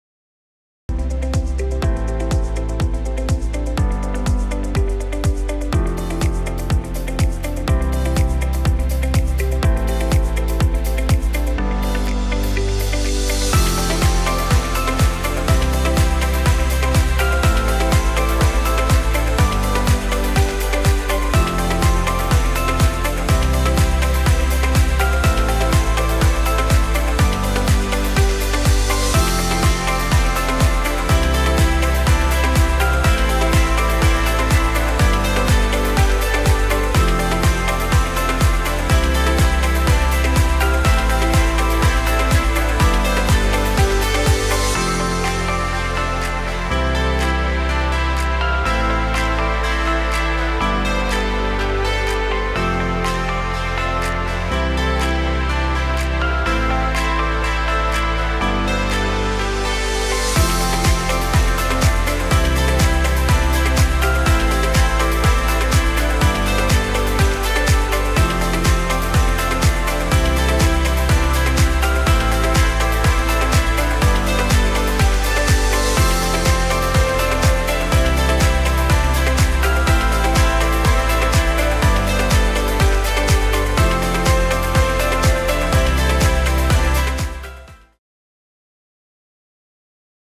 Bruce Highway (Cooroy to Curra) Section D—Time lapse of Flood Road interchange construction
The time lapse video which spans over 18 months shows the construction of the Flood Road interchange, bridge and roundabouts.